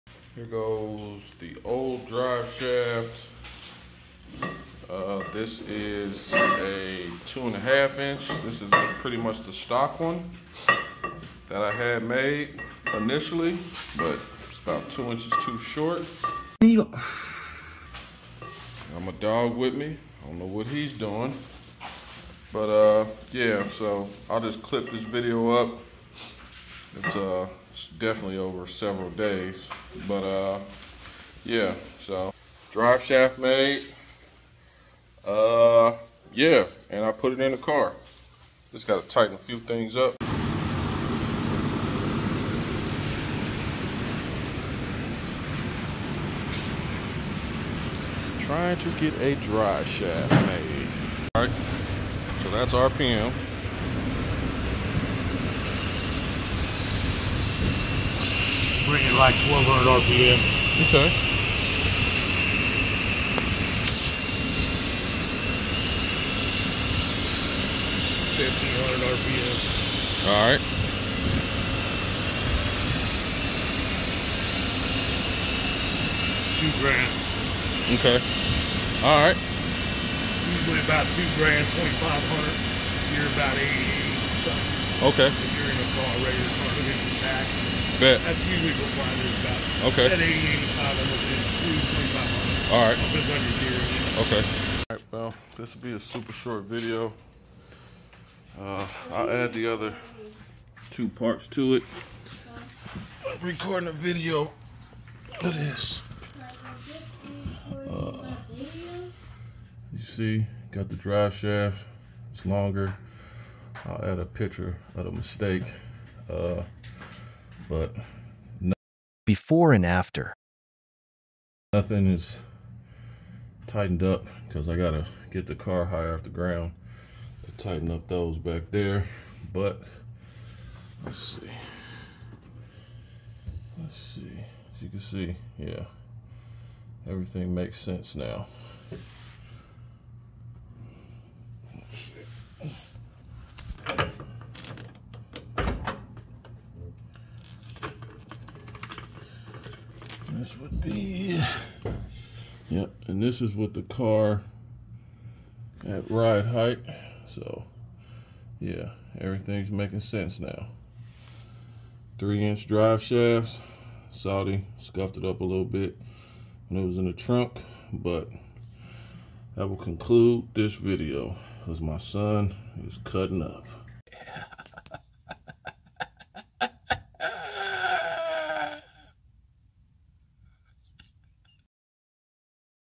WELP!!! Driveshaft got some gas station pills 🤣🤣🤣
driveshaft was about 2 inches short, so now it's 2 inches long and 1/2 bigger. Now it's a 3in driveshaft.